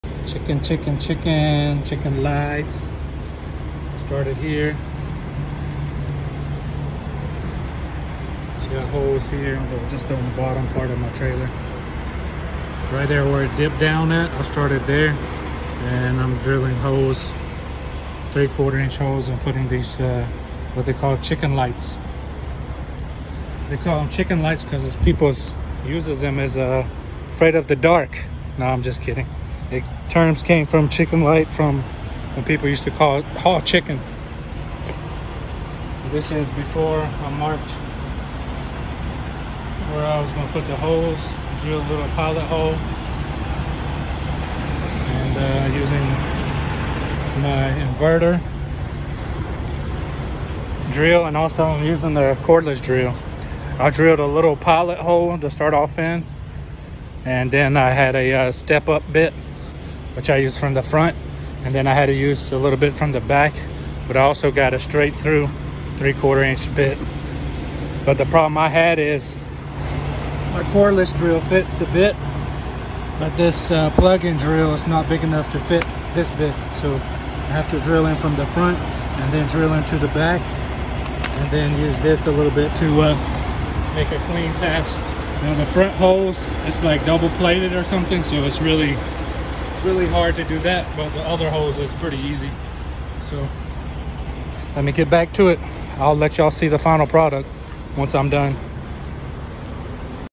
Truckin Life, How to install Chicken lights part 1!!!
Showing u how I install chicken(button) lights on my dry van trailer but will do same concept on anything u can put hole and lights. I started by putting a piece of tape in the general area I wanted to put the lights. Step back look at spacing and make sure it’s good. Then I measure center of the space when light was going and measured up 2.5” from bottom of trailer. Put a dot on the 2 lines and drilled a 3/4 hole to fit the rubber grommet and light inside.